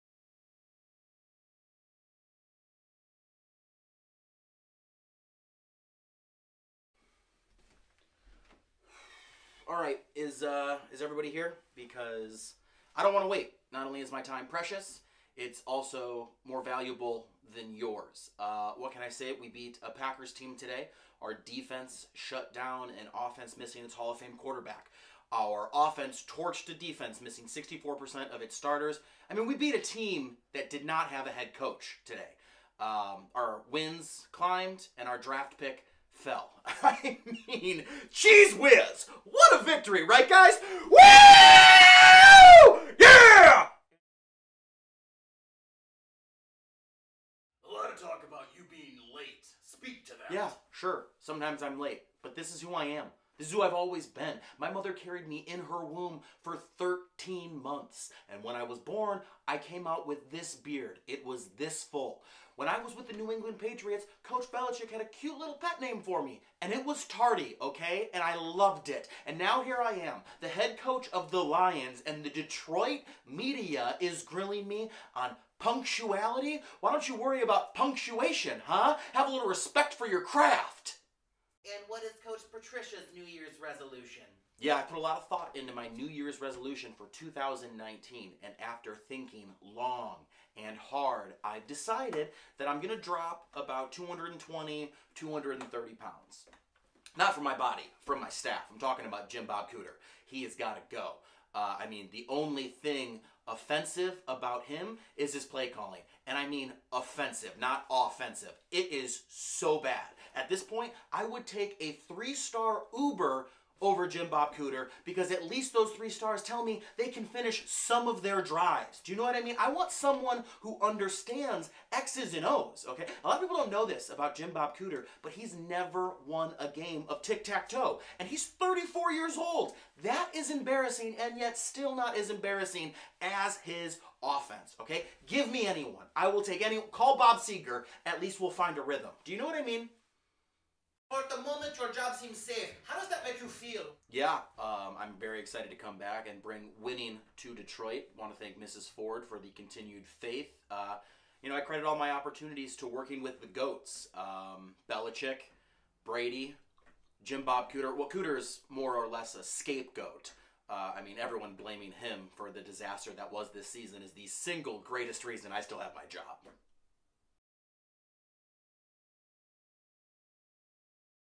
Detroit Lions @ Green Bay Packers: Matt Patricia Post Game Press Conference 2018
Coach Matt Patricia following the Detroit Lions victory over the Green Bay Packers at Lambeau Field. Topics include punctuality, his new years resolution and retaining his job.